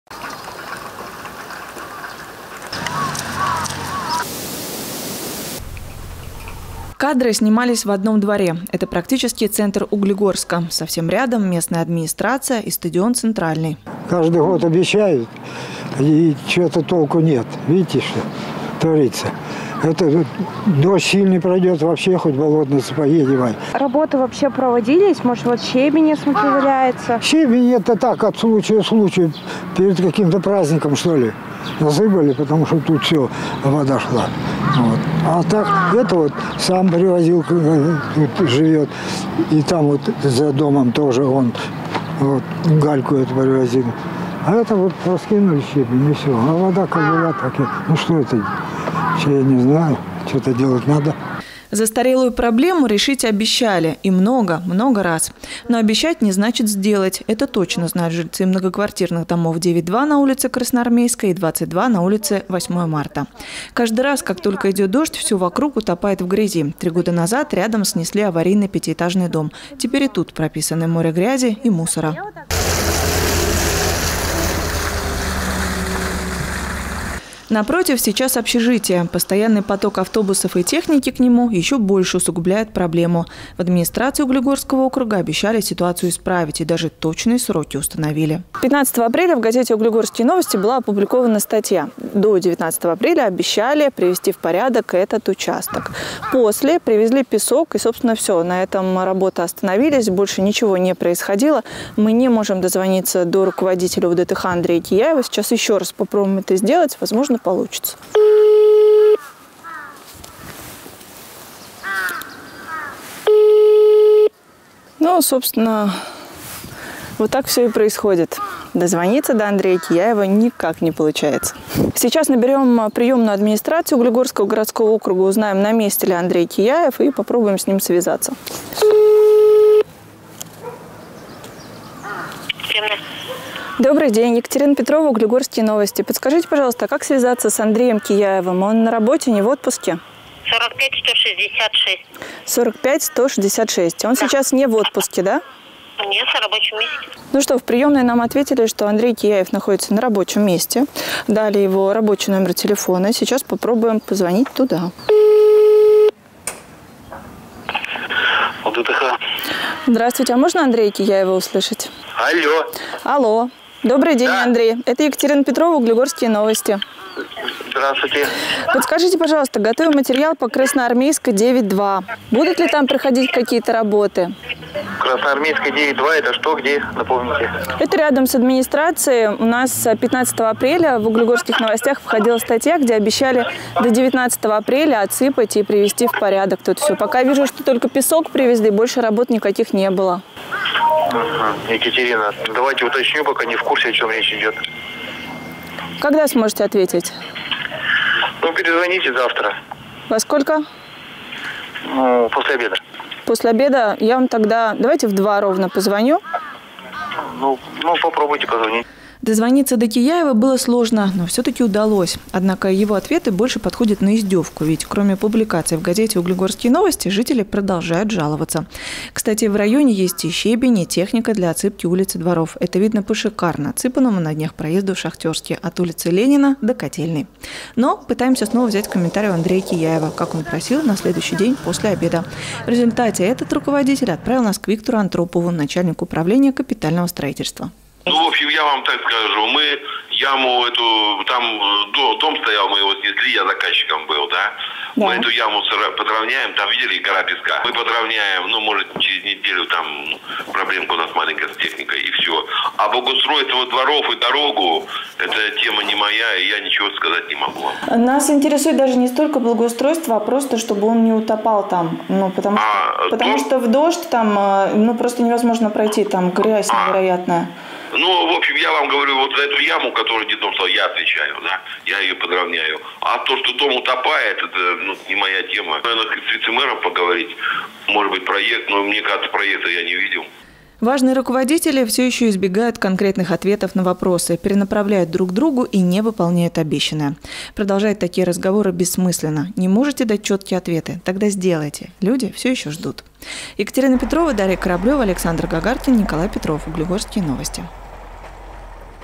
Двор позора, или Что мешает углегорским властям отсыпать щебнем одну из самых запущенных улиц?
Полтора месяца назад углегорская администрация пообещала благоустроить двор и проезды к нескольким домам в райцентре. Обещание так и не выполнено. Исполнителям проще прикидываться забывчивыми и перекладывать ответственность. А жители тем временем отчаялись.

Мы попытались достучаться до власть имущих.